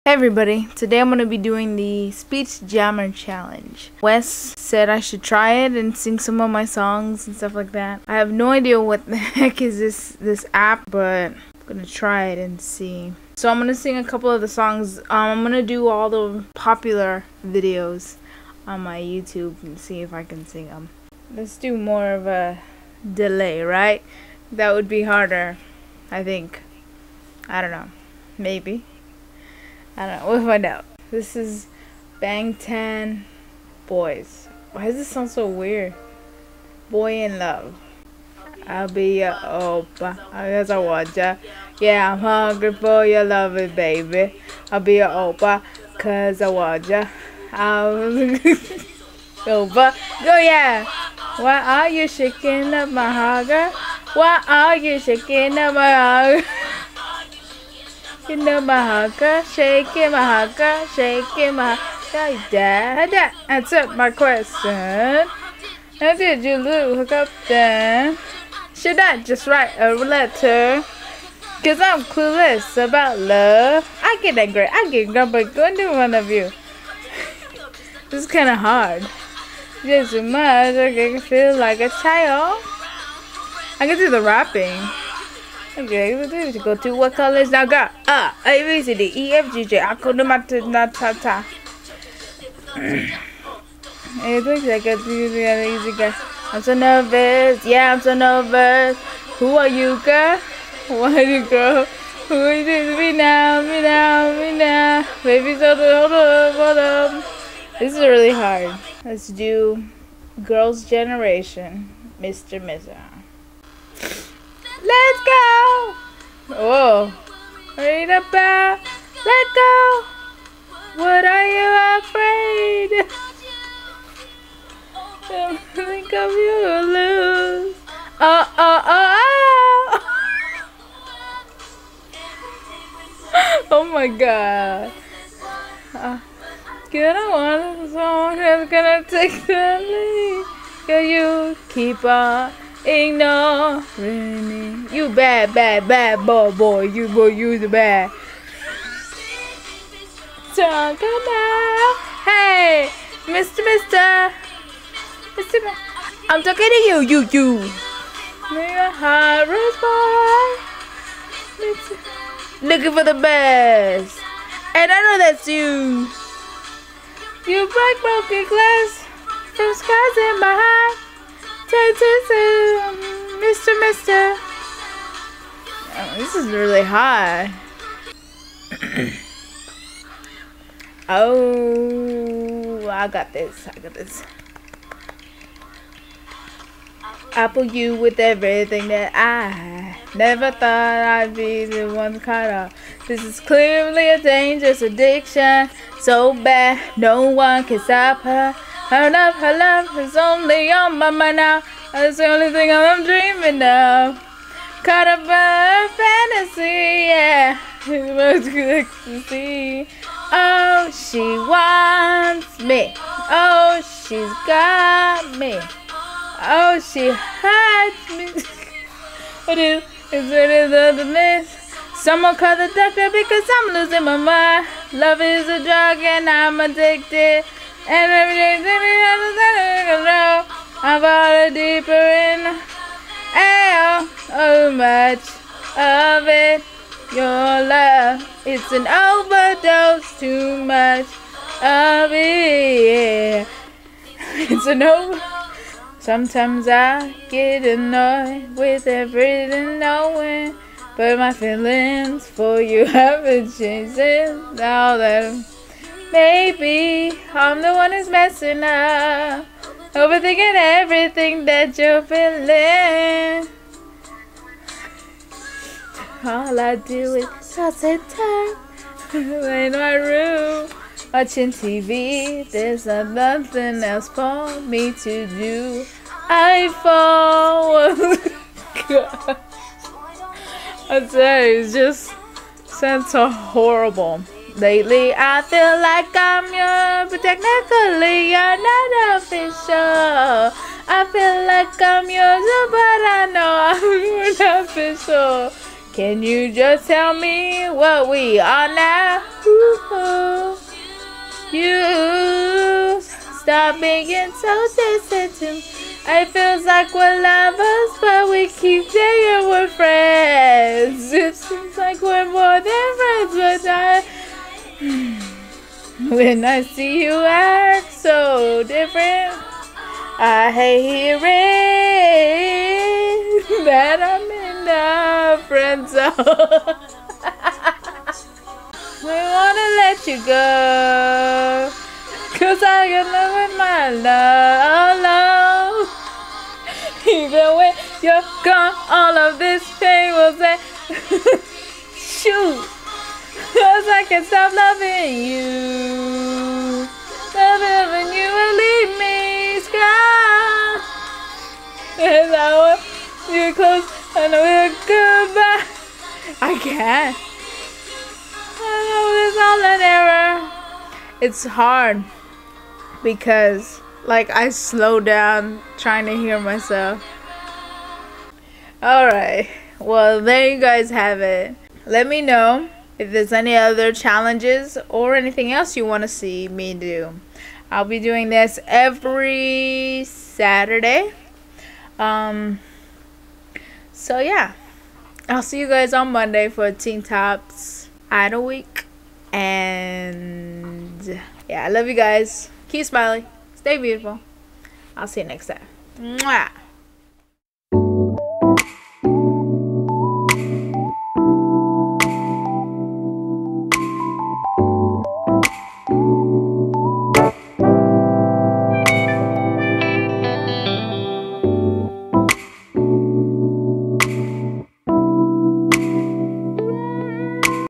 Speech Jammer Challenge
So Wes said I should try it and I got to be honest this was much harder than I expected it to be. Please forgive my off key singing hahaha. What other challenges should I do?

I love fan mail so send me some ^_^:
Mallorie Herrera